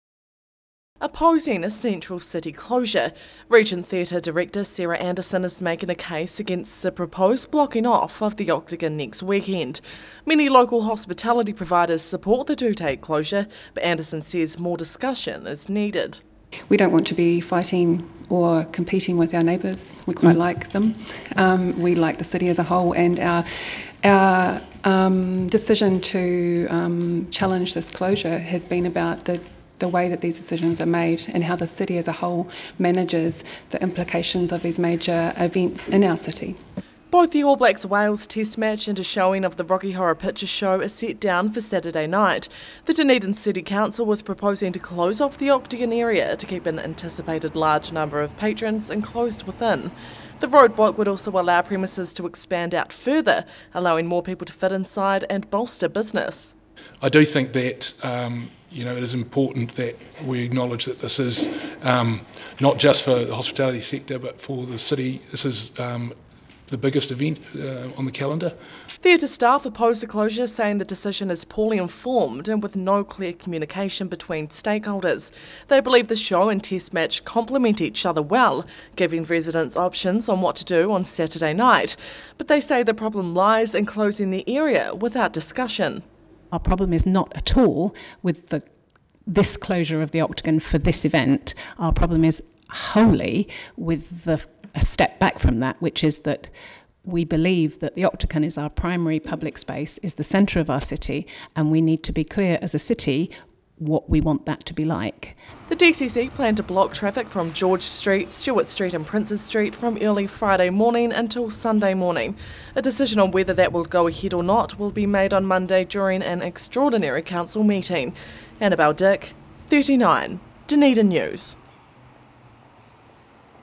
Two day Octagon road closure opposed
Plans to close off the Octagon for the All Blacks test match next weekend have hit a roadblock.

Staff at the Regent Theatre are opposing the closure, making their case to the Dunedin City Council.

And they say their main issue is the lack of communication between interested parties.